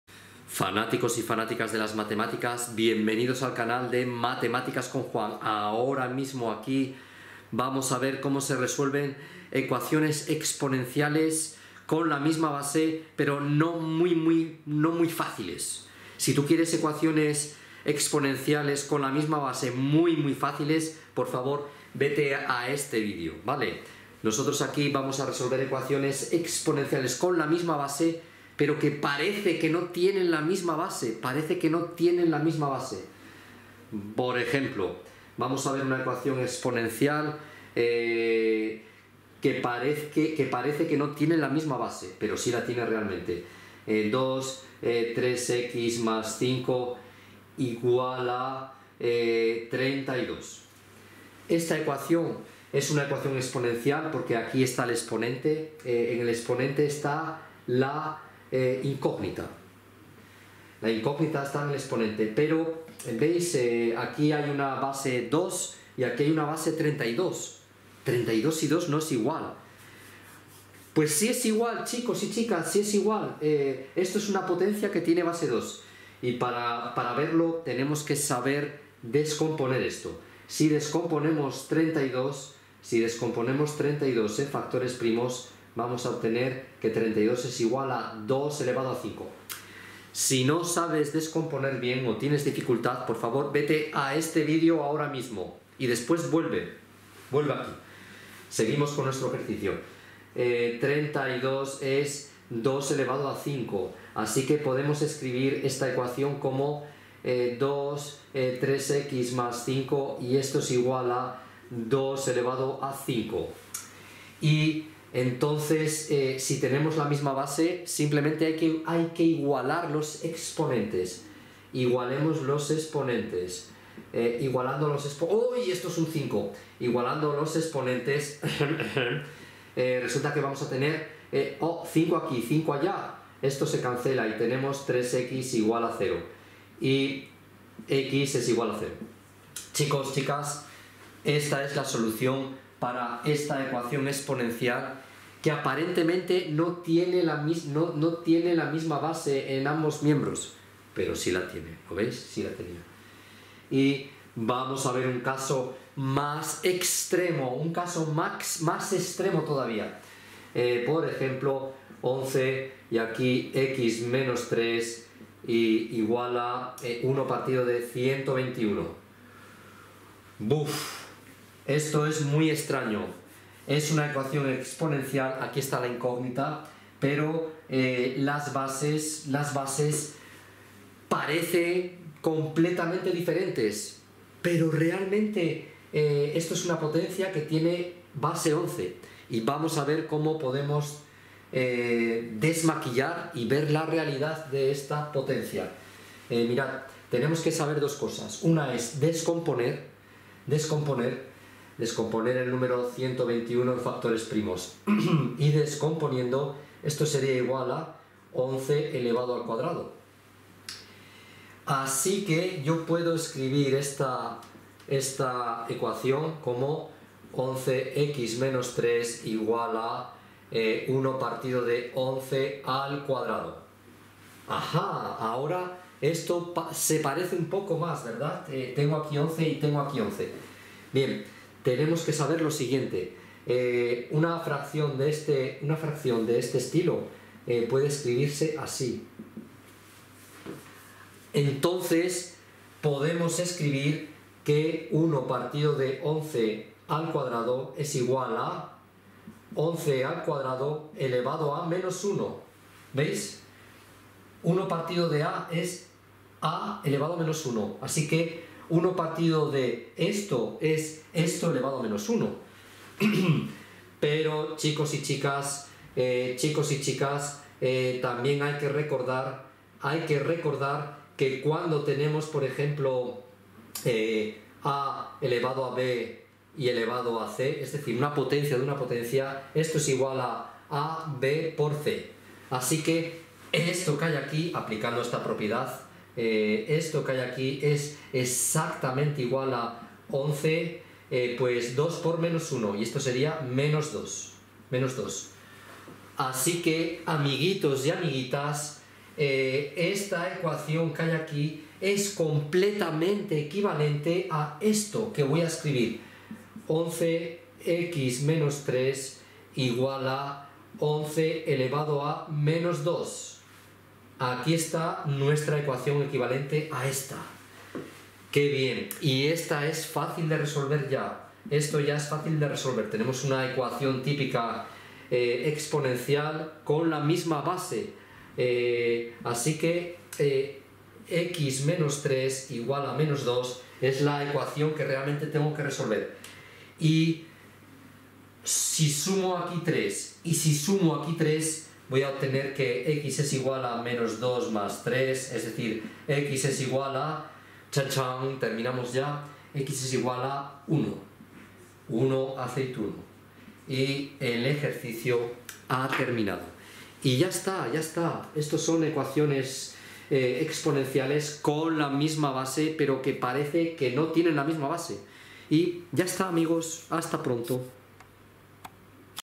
▷ Cómo resolver ecuaciones exponenciales con BASES IGUALES. Segunda clase
Resolver ecuaciones exponenciales con bases iguales es lo que vamos a hacer a continuación. Se trata de dos ecuaciones exponenciales que aparentemente no tienen la misma base, pero eso es solo una apariencia.

Venga, vamos con la primera de estas ecuaciones:

0:42 2^(3x+5)=32.

1:00 En esta ecuación, si descomponemos en factores primos el número 32 obtenemos 2^5. Entonces podemos escribir 2^(3x+5)=2^5, lo cual es equivalente a 3x+5=5, siendo la solución de esa ecuación x=0. ¡Y ya está este primer ejercicio!

2:40 Vamos con la segunda ecuación: 11^(x-3)=1/121

3:17 Lo que hay que hacer es descomponer en factores primos 121, es decir 121=11^2.

3:52 Ahora hay que recordar que (1/a)=a^(-1). Entonces es posible escribir 1/(11^2)=(11^2)^(-1)

4:59 Podemos decir que la ecuación equivalente a la dada, teniendo en cuenta todas estas consideraciones, es 11^(x-3)=11^(-2) y esto a su vez es igual a x-3=-2, lo cual es x=1

¡Hemos terminado!